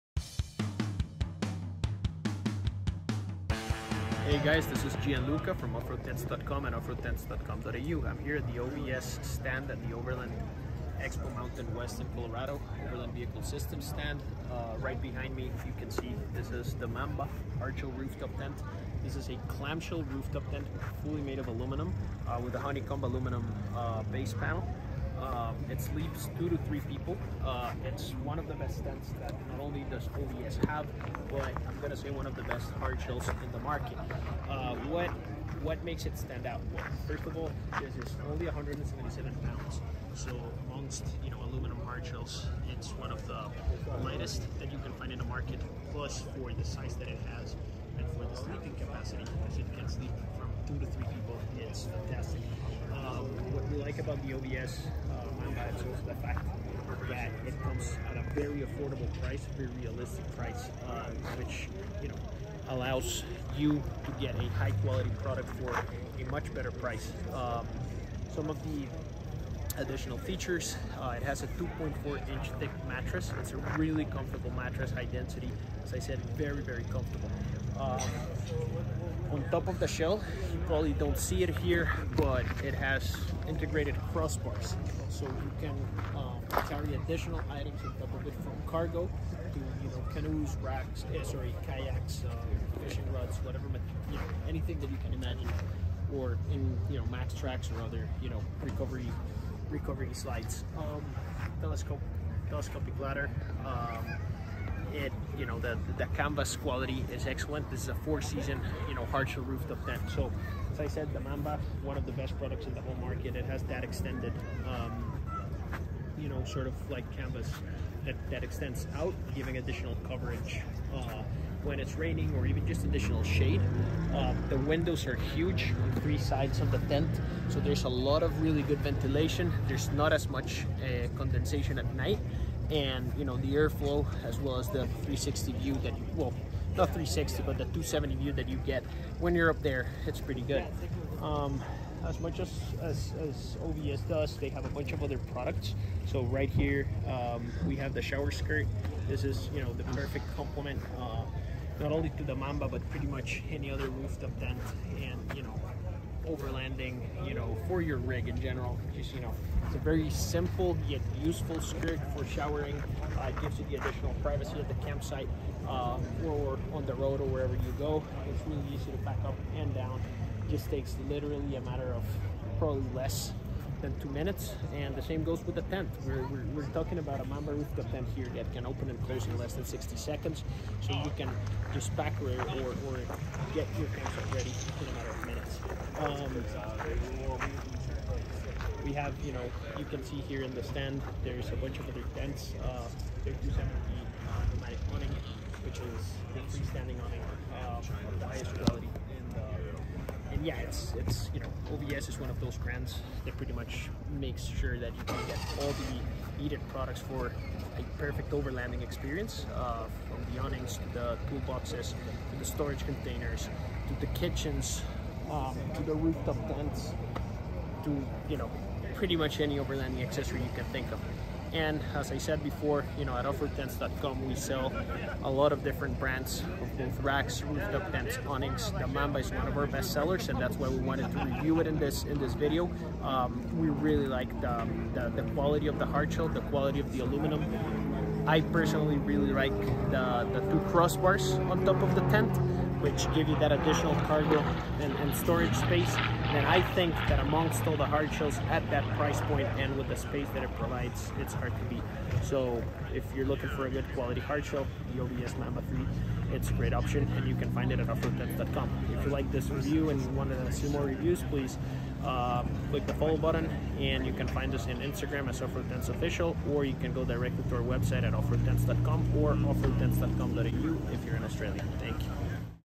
OVS Mamba Roof Top Tent Walkthrough At Overland Expo Mtn West 2023
In this video we do a walkthrough and review of the OVS Mamba 3 Person Roof Top Tent. We also get a sneak peak of the awesome Ford F150 that OVS had in their stand at the Overland Expo Mountain West 2023 show.

The Overland Vehicle Systems Mamba Clamshell roof top tent is one of those tents that offers incredible looks, toughness and versatility. It comes with 2 integrated cross bars and offers a ver low profile of just 8" when closed.

The Mamba is also an aluminum hardshell roof top tent, this means that it is relatively lightweight and can fit a lot of different vehicles. You only need a roof rack to mount it and you're all set.

On this video we also go over the Nomadic Car Side Shower Room, ideal to pair with the Kakadu Outback shower (featured on our previous video uploaded to our channel), and the OVS 270 Awning. That F150 was a complete Overlanding monster.